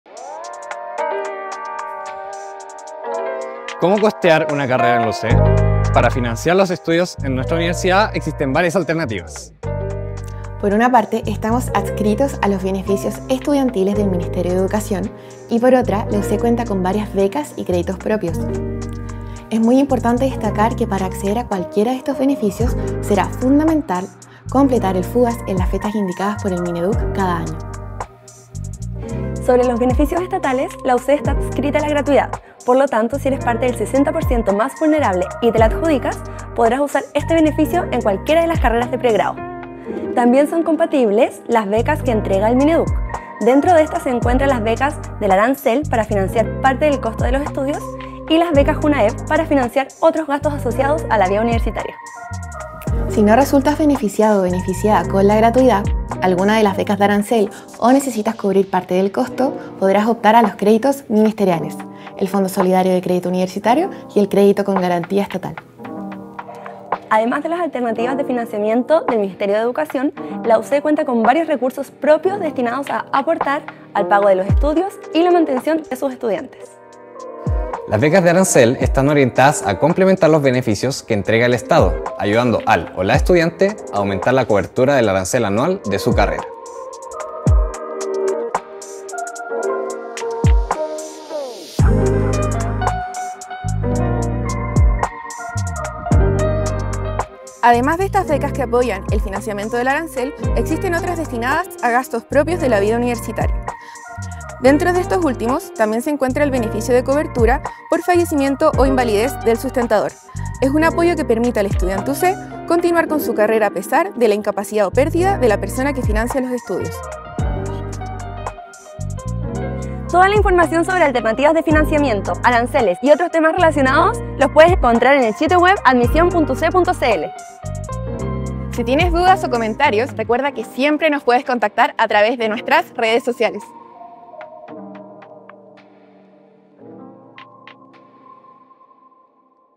¿Cómo financiar una carrera en la UC?
Revisa este video con toda la información sobre Financiamiento en la UC, y recuerda que todo está disponible en nuestro sitio web admision.uc.cl